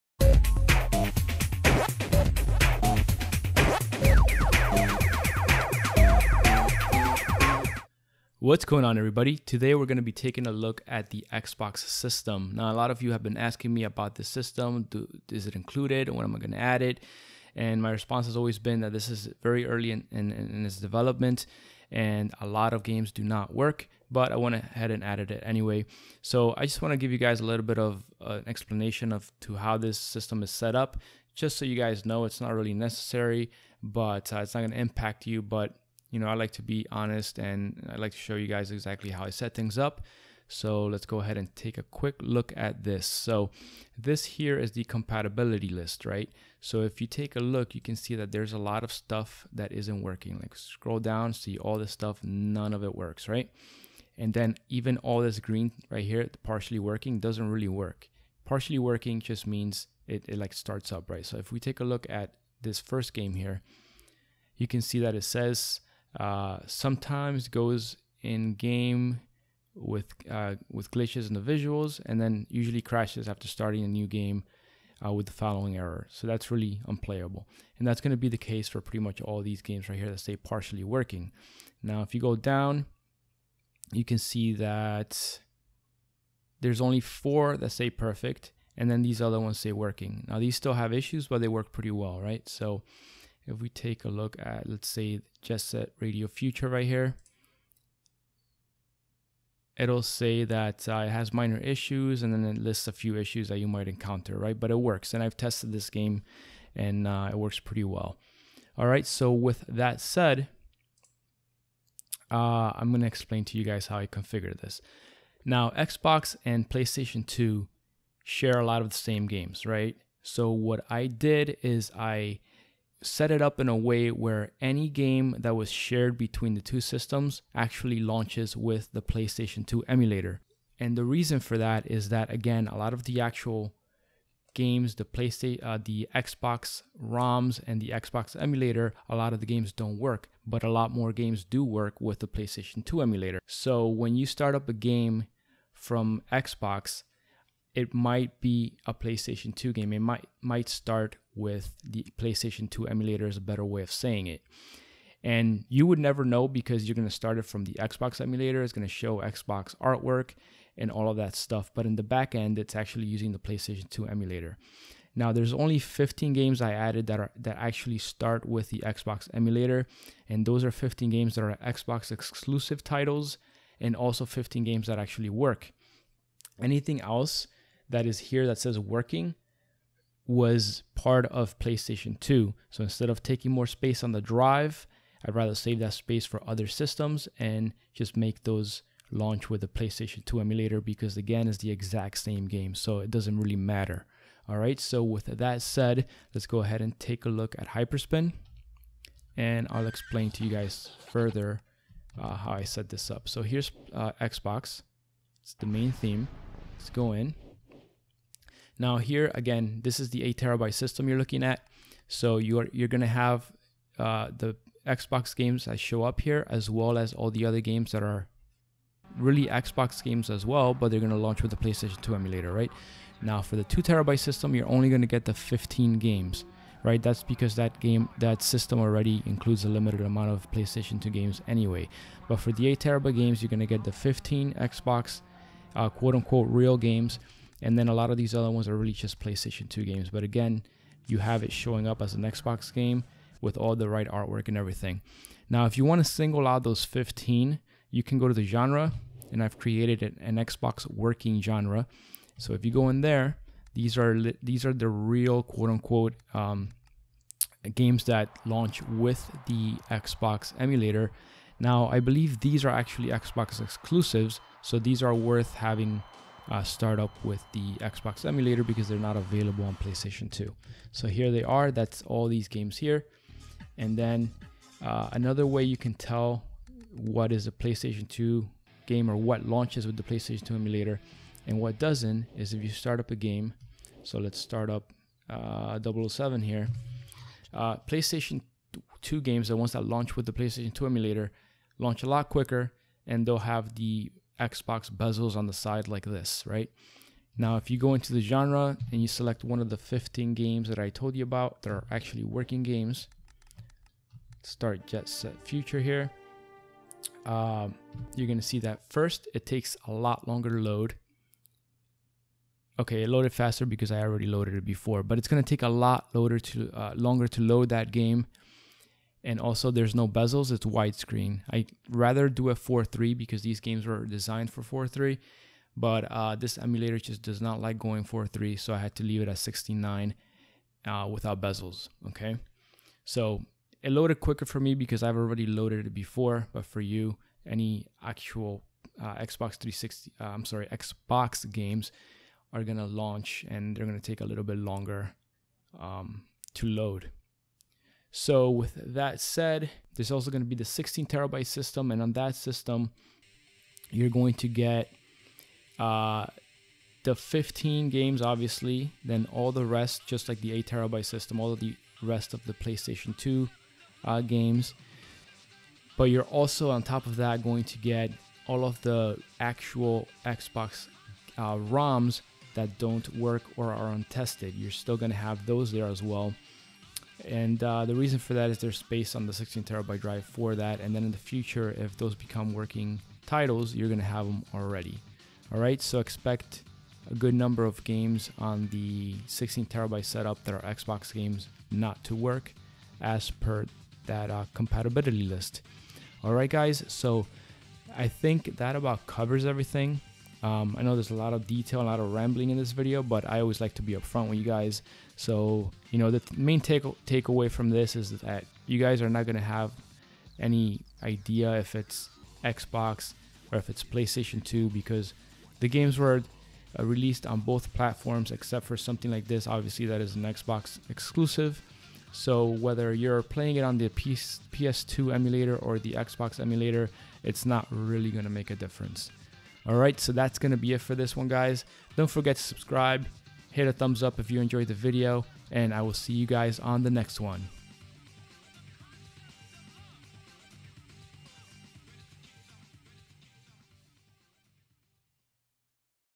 Hyperspin Video Manual: Microsoft Xbox
Detailed explanation about how the Microsoft Xbox emulator is configured on the Arcade-One systems.